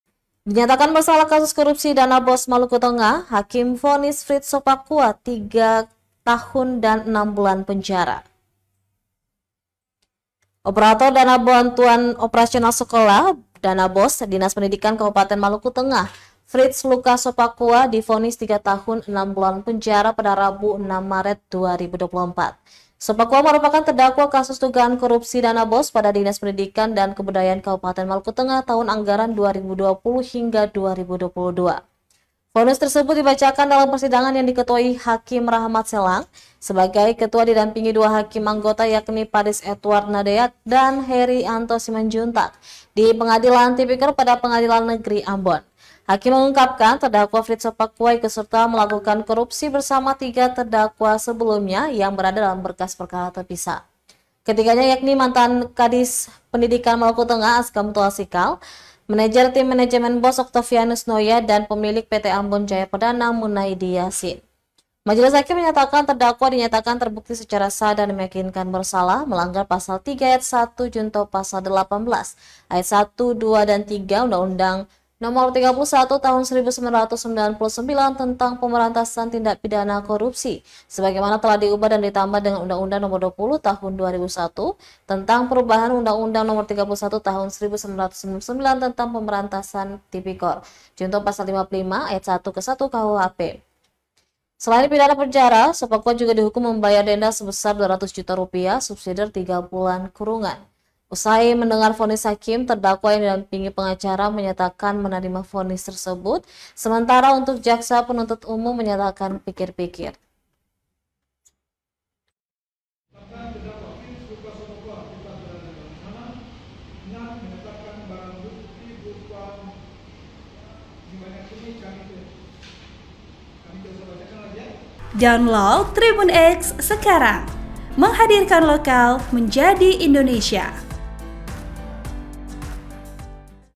Dinyatakan Bersalah Kasus Korupsi Dana BOS Malteng, Hakim Vonis Fritz Sopacua 3,6 Tahun Penjara
OA,Laporan Wartawan TribunAmbon.com, Tanita Pattiasina
AMBON, TRIBUNAMBON.COM - Operator Dana Bantuan Operasional Sekolah (BOS) Dinas Pendidikan Kabupaten Maluku Tengah Frits Lukas Sopacua divonis 3 tahun 6 bulan penjara, Rabu (6/3/2024).
Sopacua merupakan terdakwa kasus dugaan korupsi dana BOS pada Dinas Pendidikan dan Kebudayaan Kabupaten Maluku Tengah Tahun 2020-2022.
Vonis tersebut dibacakan dalam persidangan yang diketuai Hakim Rahmat Selang sebagai ketua didampingi dua Hakim anggota yakni Paris Edward Nadeak dan Herry Anto Simanjuntak, di Pengadilan Tipikor pada Pengadilan Negeri Ambon, Rabu (6/3/2024).
Hakim mengungkapkan, Terdakwa Frits Sopacua ikut serta melakukan korupsi bersama tiga terdakwa sebelumnya (berkas perkara terpisah).
Ketiganya yakni mantan Kadis Pendidikan Malteng Askam Tuasikal; Manajer Tim Manajemen BOS, Oktovianus Noya dan Pemilik PT Ambon Jaya Perdana, Munaidi Yasin.
Majelis Hakim menyatakan terdakwa dinyatakan terbukti secara sah dan meyakinkan bersalah melanggar Pasal 3 ayat (1) Jo Pasal 18 ayat (1), ayat (2) dan ayat (3) Undang-undang (UU) Nomor 31 Tahun 1999 Tentang Pemberantasan Tipikor sebagaimana telah diubah dan ditambah dengan UU Nomor 20 Tahun 2001 tentang Perubahan Undang-undang Nomor 31 Tahun 1999 Tentang Pemberantasan Tipikor Jo Pasal 55 ayat (1) Ke-1 KUHP sebagaimana dalam Dakwaan Primair Penuntut Umum.
Selain pidana penjara, Sopacua juga dihukum membayar denda sebesar Rp. 200 juta rupiah subsider 3 bulan kurungan.
Usai mendengarkan Vonis Hakim, Terdakwa yang didampingi pengacara menyatakan menerima vonis tersebut sementara JPU menyatakan pikir pikir.
Diketahui, Vonis tersebut lebih ringan dari tuntutan JPU Maluku Tengah yang menghendaki agar terdakwa dihukum empat tahun penjara.(*)